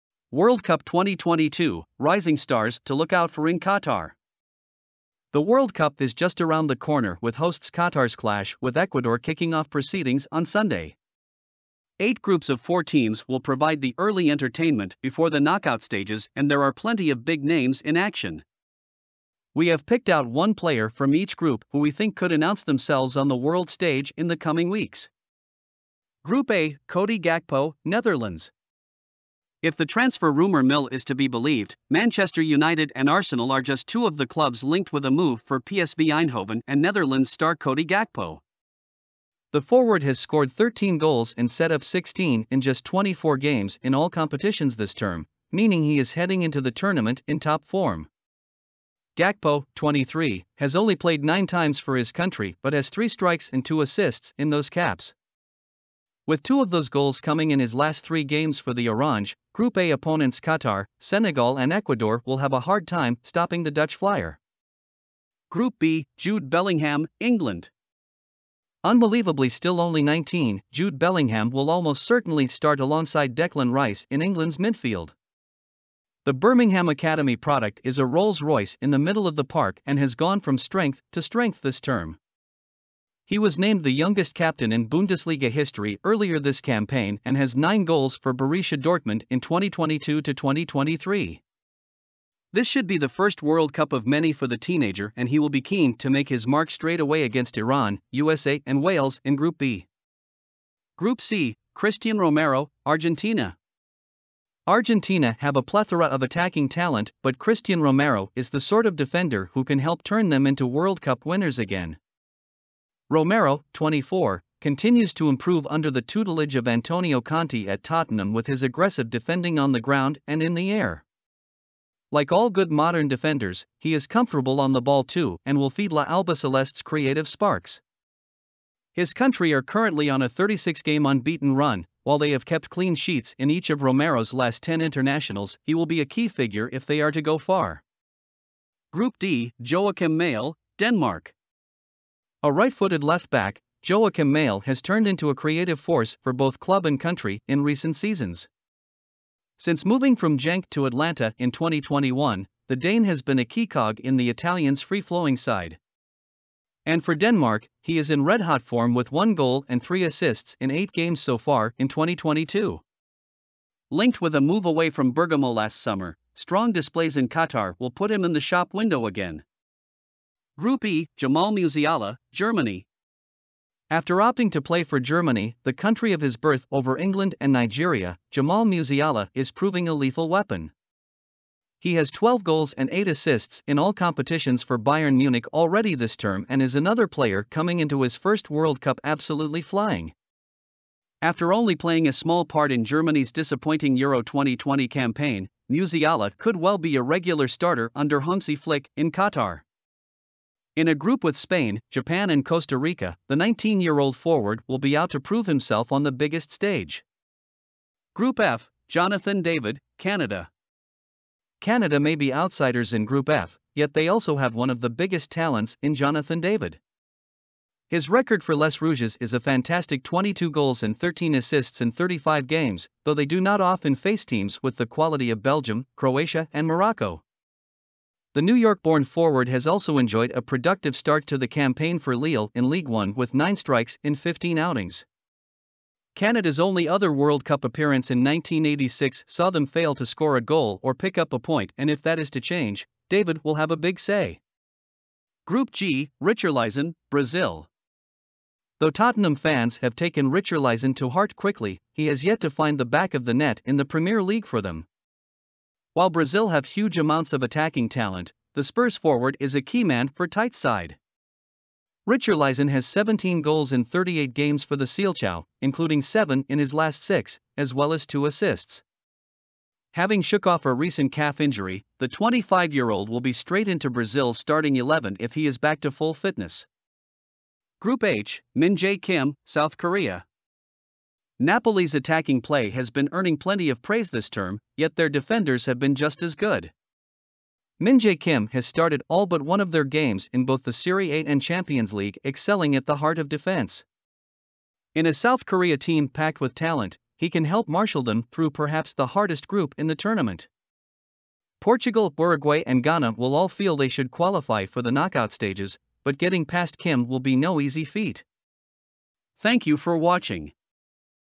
Qatar World Cup 2022: Rising stars to look out for in Qatar
WorldCup 2022: Rising stars to look out for in Qatar
Group A: Cody Gakpo (Netherlands)
Group B: Jude Bellingham (England)
Group C: Cristian Romero (Argentina)
Group D: Joakim Maehle (Denmark)
Group E: Jamal Musiala (Germany)
Group F: Jonathan David (Canada)
Group G: Richarlison (Brazil)
Group H: Min-Jae Kim (South Korea)